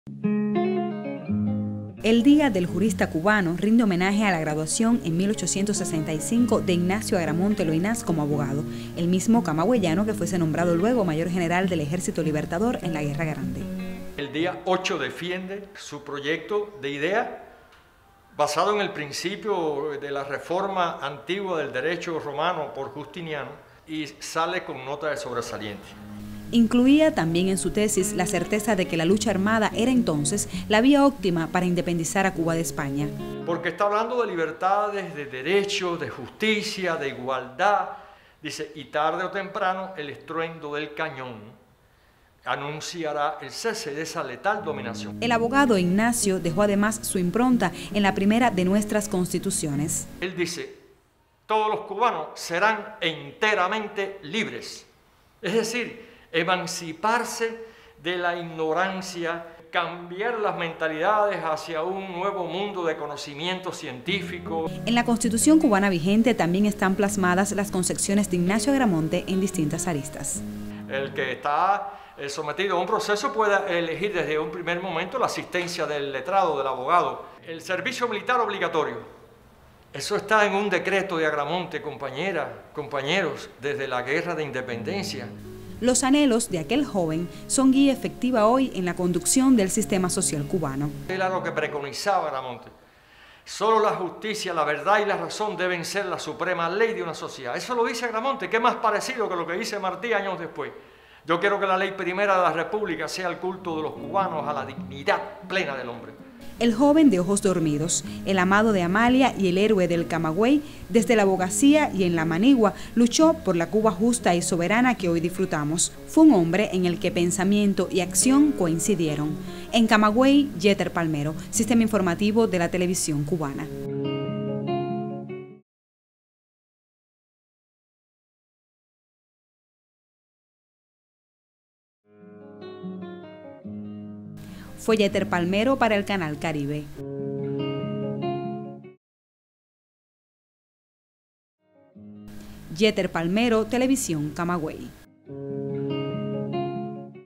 El Día del Jurista Cubano rinde tributo al legado del abogado camagüeyano Ignacio Agramonte
En Cuba cada 8 de junio se celebra el Día del Jurista para recordar que en igual fecha, pero en 1865, Ignacio Agramonte Loynaz defendió su tesis para recibirse como Licenciado en la Facultad de Derecho de la Universidad de La Habana.
Sobre la impronta del pensamiento de este singular patriota camagüeyano en las leyes cubanas, trata el siguiente material periodístico...